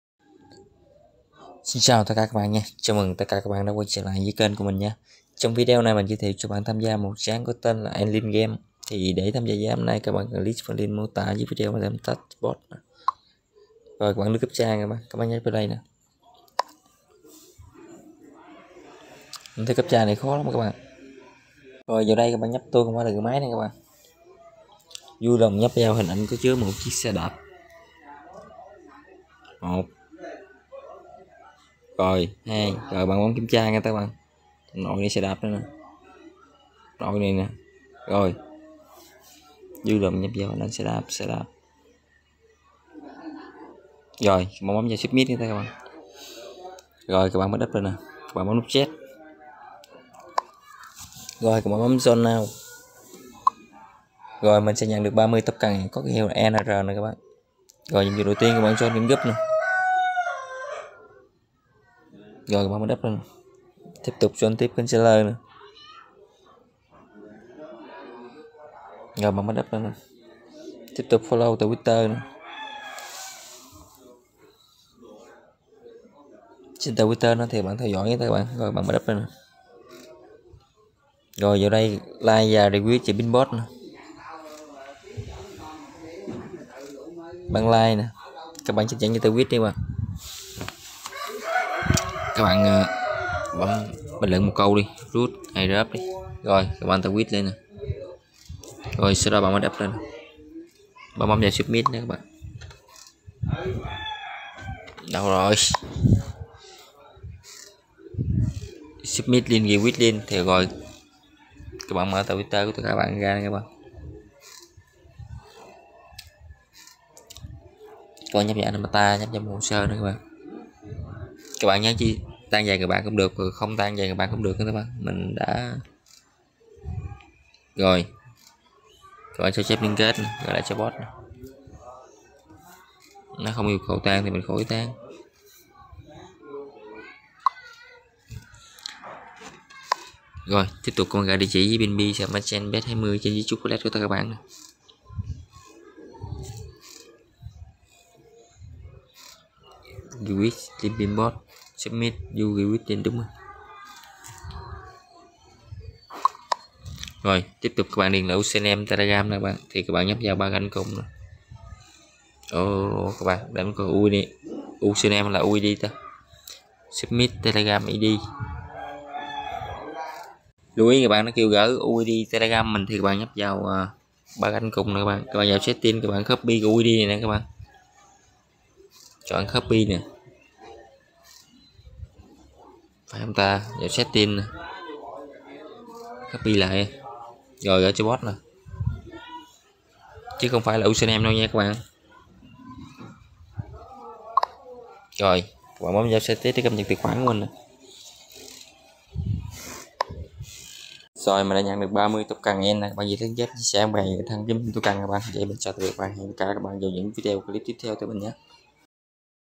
hướng dẫn nhận 300 ENG - Erin Game Airdrop
💧 Erin Game @Airdrop 💧

🏆 Task:          ➕  300 ENG

👨‍👩‍👧 Referral:   ➕ 50 ENG

💠 Join  Erin Game Telegram group & Telegram channel.
💠 Follow  Erin Game on Twitter and retweet the pinned post.
💠 Enter your information to the airdrop bot.

🔐 All airdrop steps should be completed.

🗞 Notes:  You should have at least 10 referrals to get airdrop reward.

⏳ Distribution date: Every Sunday.